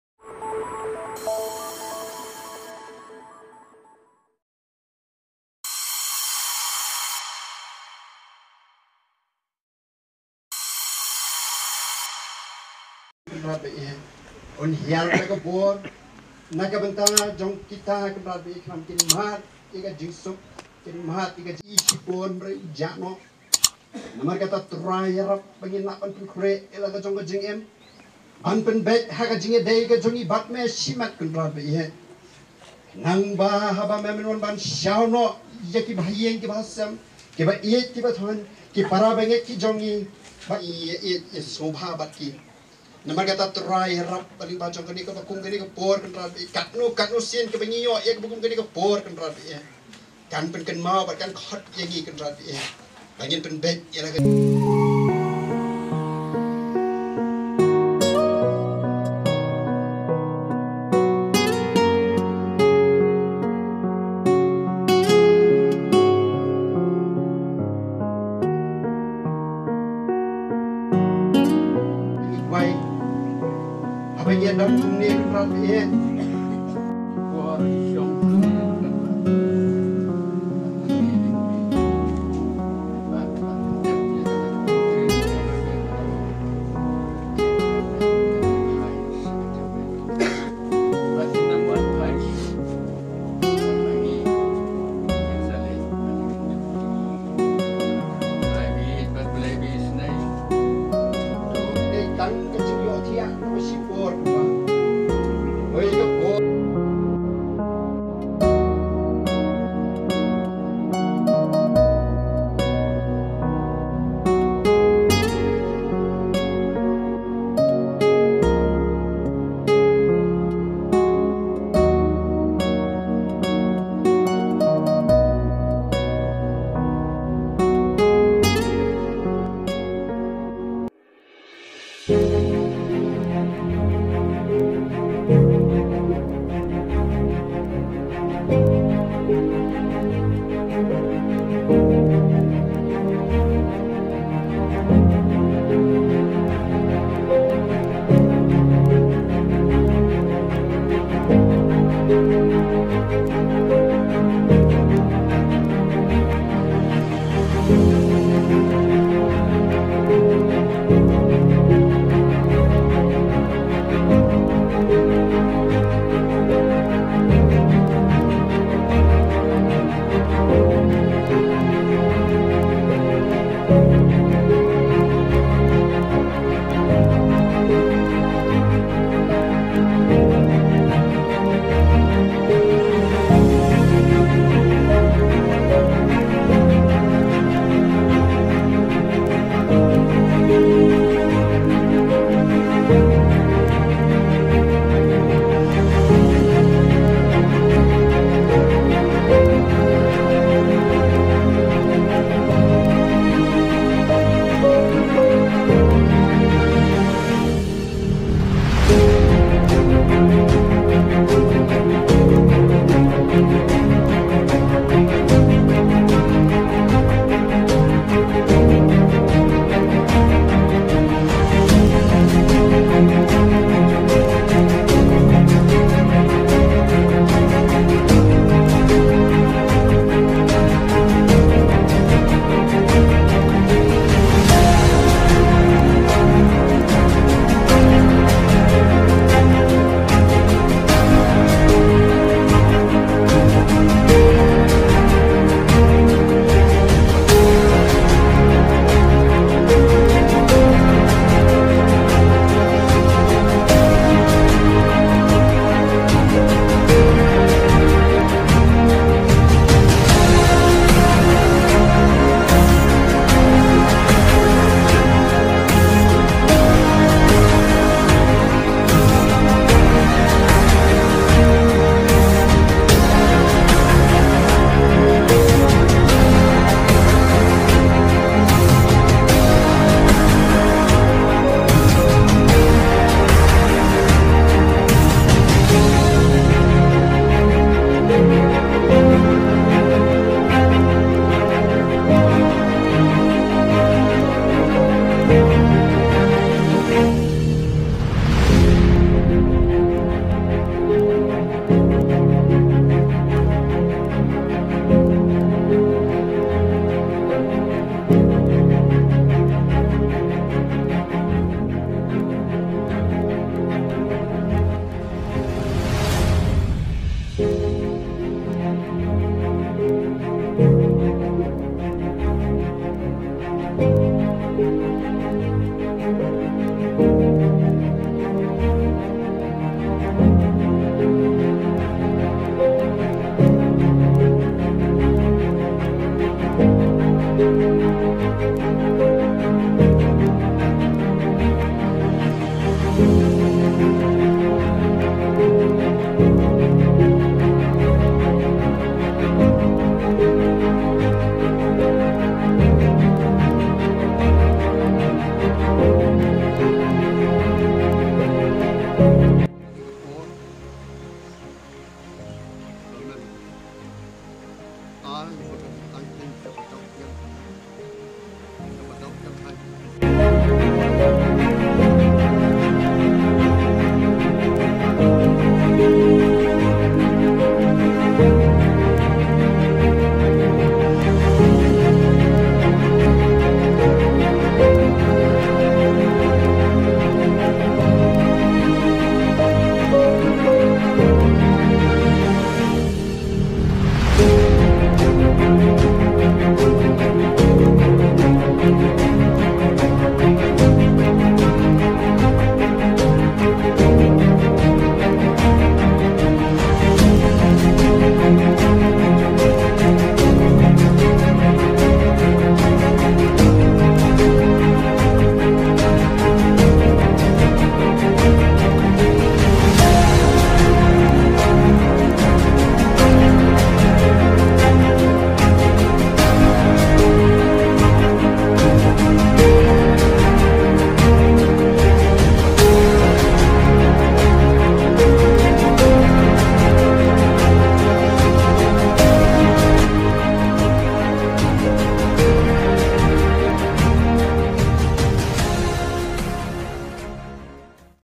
|| LATEP DON BUROM IA I MEIIEID (L) J.SURONG HAKA 16.10.2021 HA U ||
LA tep don burom ia i meiieid j.surong haka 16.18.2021 ha u lum jingtep balang Presbyterian mawiong Shillong .🙏🙏🙏🙏🙏🙏🙏🙏🙏